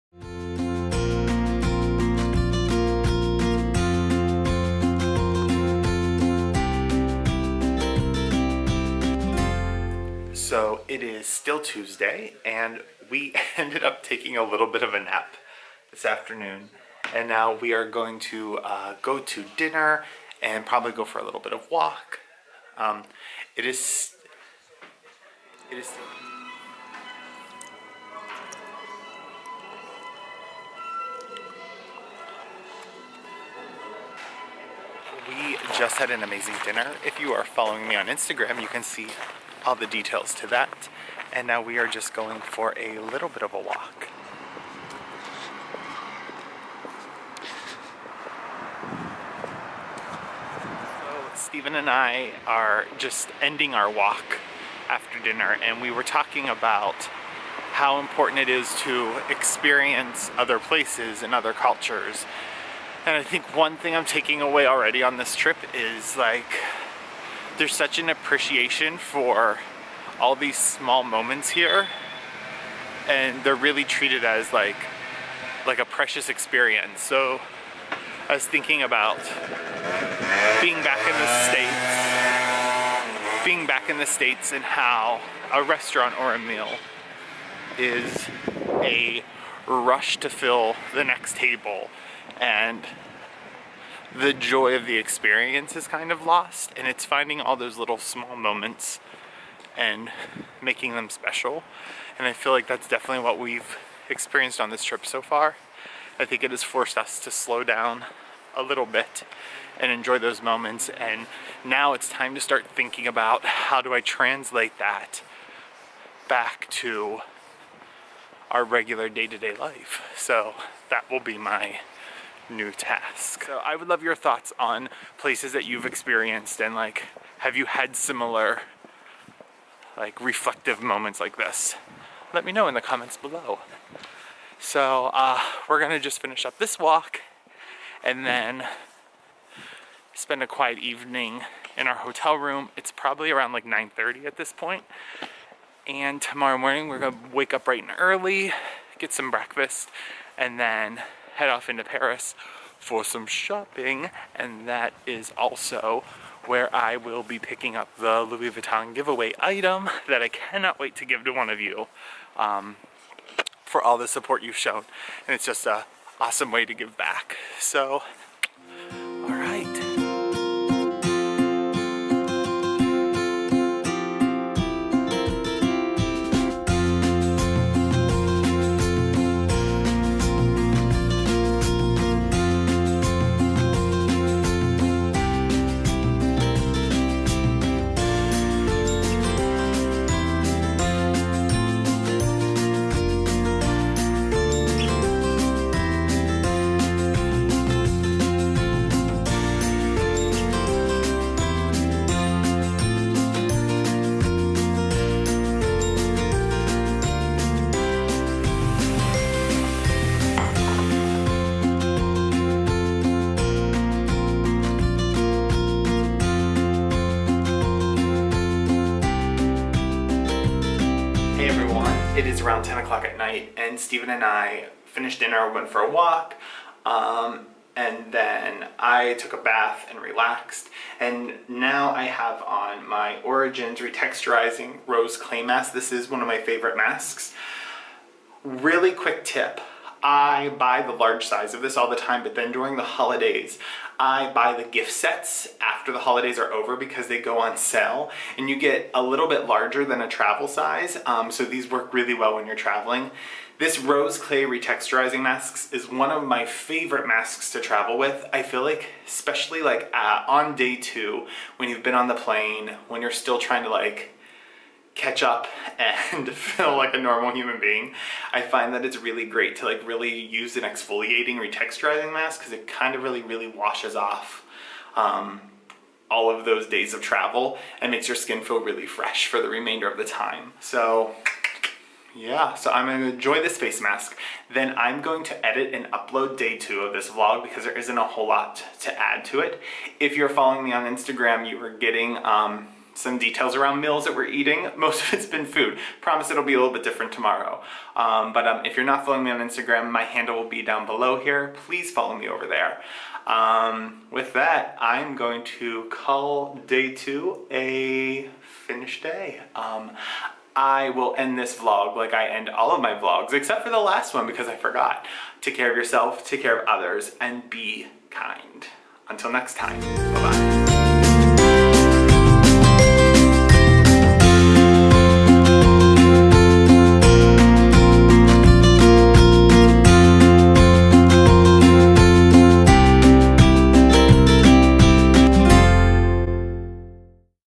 TRAVELOG: Versailles, Reflective Walk, Louis Vuitton Giveaway, and the End of Day Deux
Here is the completion of day deux. It was a beautiful end to a beautiful day. I feel so incredibly blessed to share this experience with all of you. Thank you for your support. If you would like to still enter the Louis Vuitton giveaway I've decided to extend it until the I return from France (2.24.17). All the details are in the video linked below:

Products mentioned below
Origins Rose Clay Mask

Equipment Used
Vlogging Camera/Sony A5100

Shooting Camera/Canon T5i

Car Camera/Canon Vixia Mini X

Lighting:/Limostudio (2)